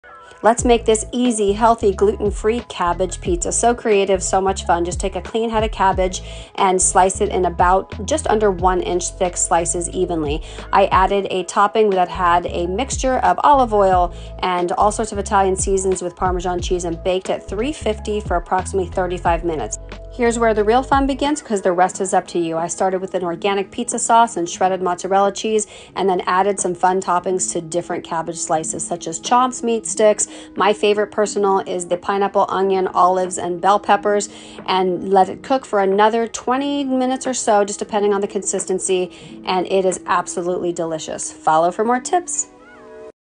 Cabbage Pizza
Sometimes all it takes for me, is the word pizza, and I am on board. Presenting this easy cabbage pizza recipe.

Start by washing a medium head of cabbage and slice into 6 even slices just under 1” thick.

Depending how soft you want your cabbage, lightly coat both sides with a little bit of olive oil, and bake for about 10 minutes at 350 degrees F.

Or you can skip that step and coat the top only with olive oil, your fave Italian seasonings, garlic and Parmesan cheese and bake for 35 minutes at 350.

I tried the first method because I was worried the cabbage wouldn’t get soft enough. Next time I will try the second step.

From here the creativity is all up to you. I used chops meat sticks on a few, and did my favorite mixture of pineapple, onion, and veggies on some of the others. Or just plain cheese.

Don’t forget to add some oregano or Italian seasoning and/or basil if you wish.

Then finish baking for another 10 to 20 minutes according to your preference.

Honestly, I didn’t even miss the pizza dough. That’s saying a lot for me! So delish! I would love to see what you make. Tag me with pizza pics.